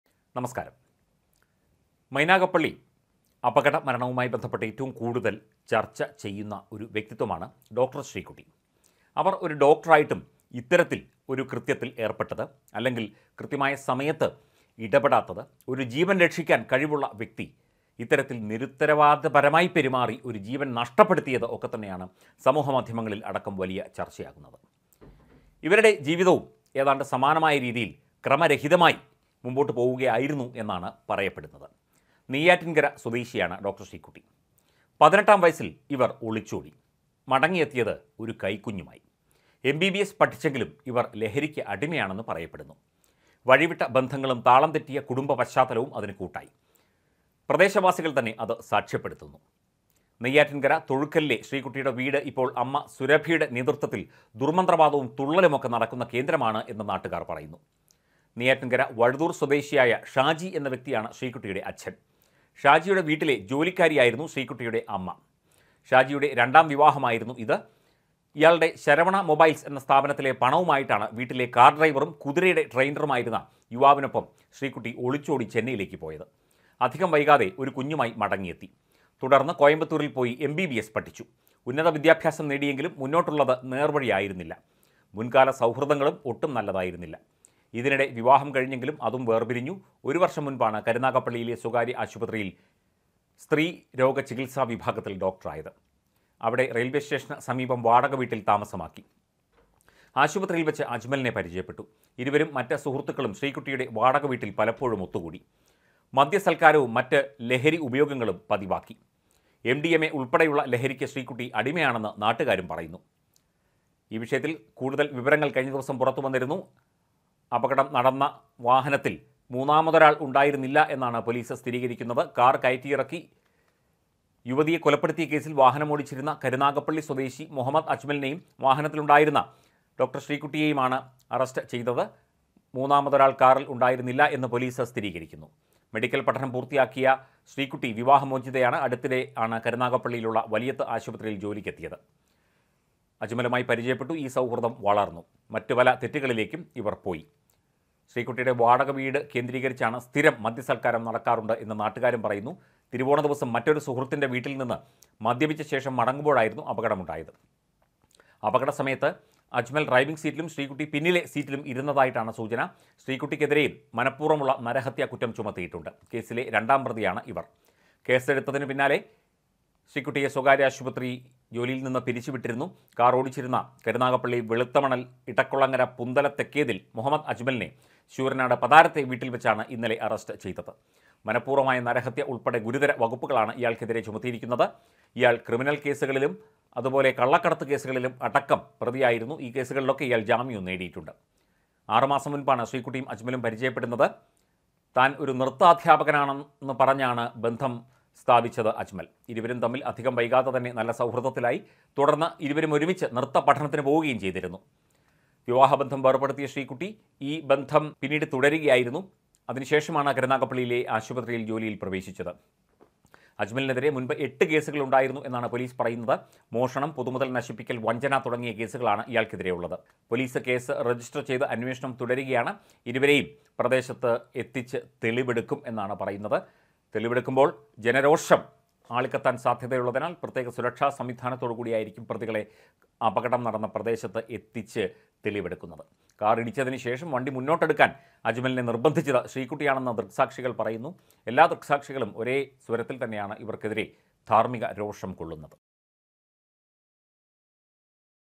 ശ്രീക്കുട്ടിയുടെ അമ്മ സുരഭിയുടെ നേതൃത്വത്തില്‍ ദുര്‍മന്ത്രവാദവും തുള്ളലും | mainagapally
18-ാം വയസില്‍ ഒളിച്ചോട്ടം. മടങ്ങിയെത്തിയത് കൈകുഞ്ഞുമായി. എം.ബി.ബി.എസ് പഠിച്ചെങ്കിലും ലഹരിക്ക് അടിമ. വഴിവിട്ട ബന്ധങ്ങളും താളംതെറ്റിയ കുടുംബ പശ്ചാത്തലവും അതിന് കൂട്ടായി. പ്രദേശവാസികള്‍ അത് സാക്ഷ്യപ്പെടുത്തുന്നു.